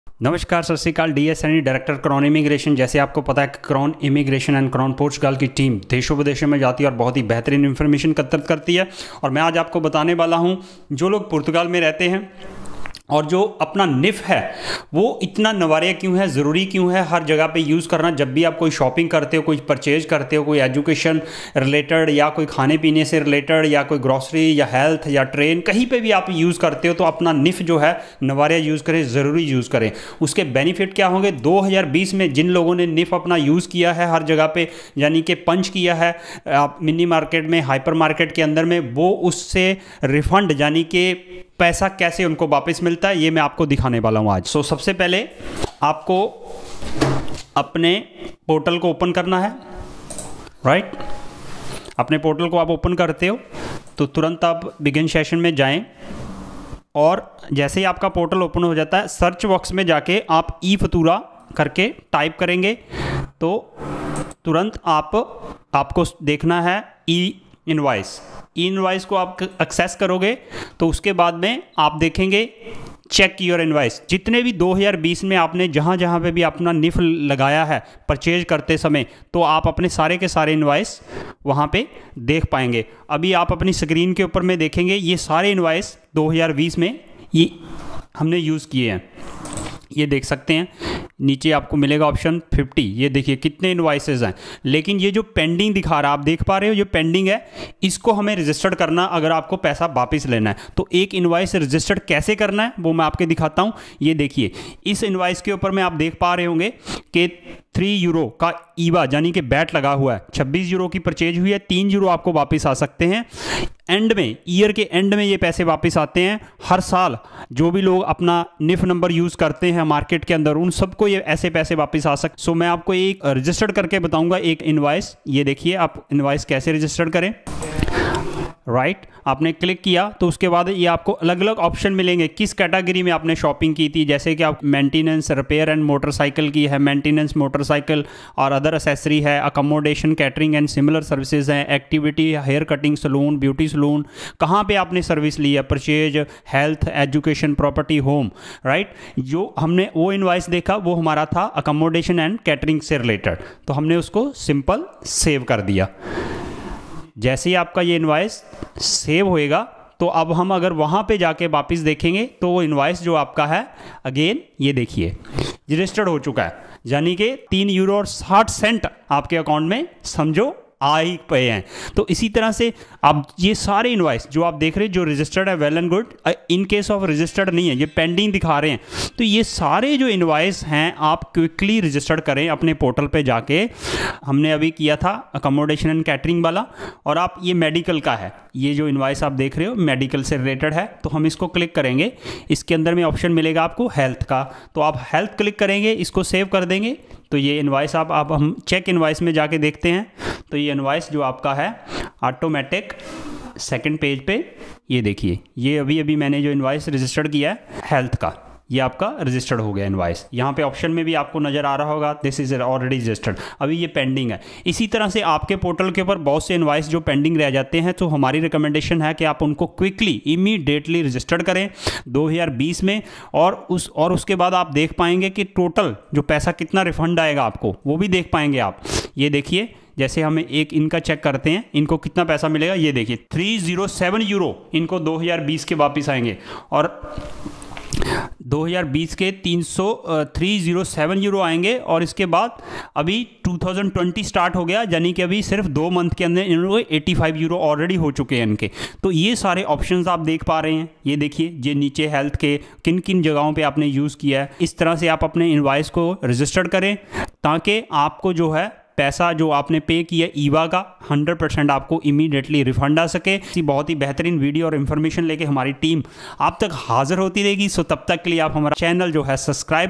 How To Registered E-Invoice And Claim VAT Refund
How to Portugal VAT refund claim as a consumer, suggested registered your e-invoices of 2020 urgent basis for the IVA refund.

WHY ASK FOR AN INVOICE?
When it requires an invoice, it guarantees that the taxes we pay are delivered to the State.

It is a civic duty that increases justice, contributing to the fight against fraud and tax evasion. It is not fair to pay more taxes because taxpayers (citizens or companies) do not fulfil their tax obligations.

Benefit for consumer asking the Invoices form vender?
Lower-income get IVA refund benefit at the end of years before filling IRS.